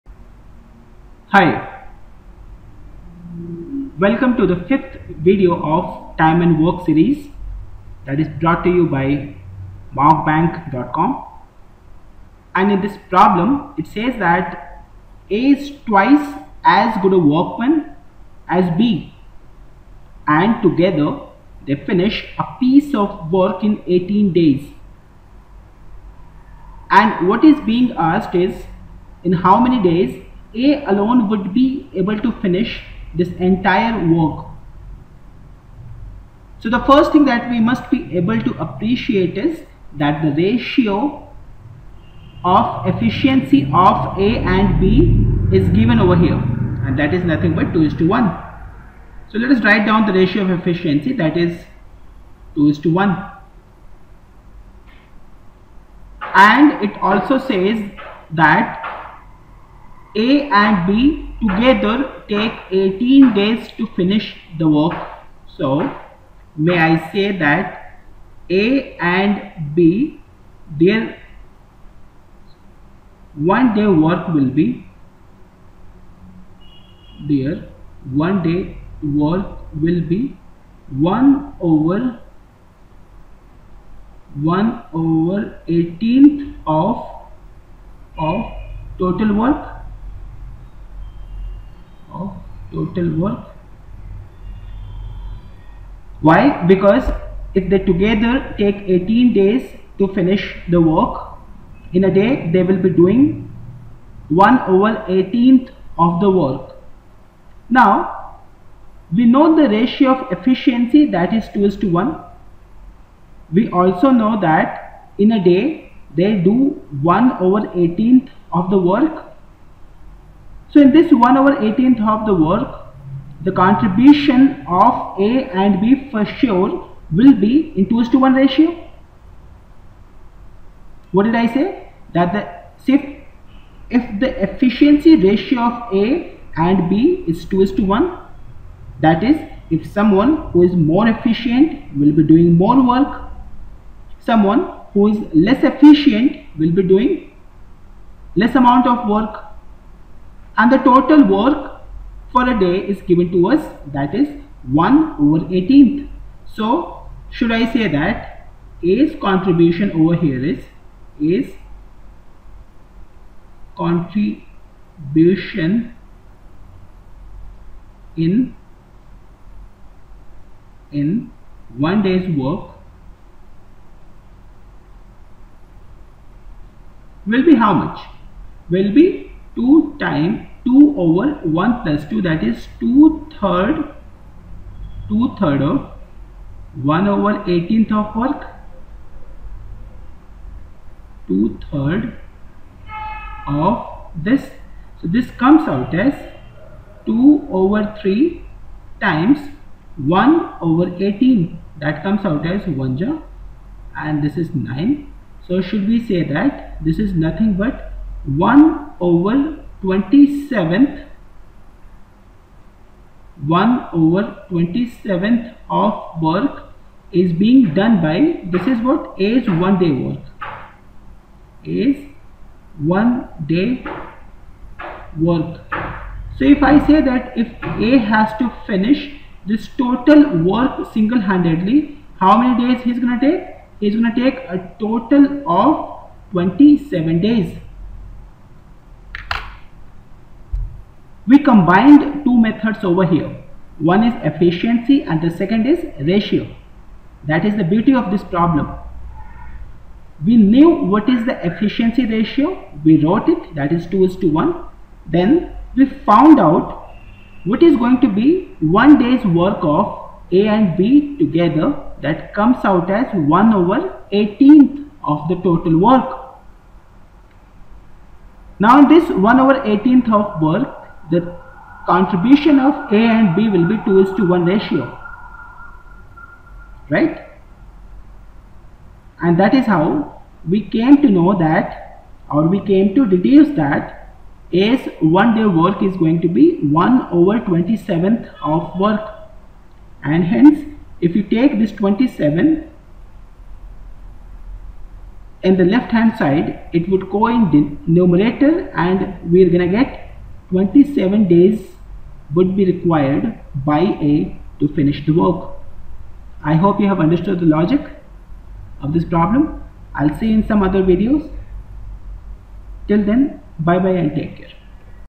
Time and work Problem Solving Approach 5
This video is for Time and work. This video consist illustrative examples and easy steps to solve the questions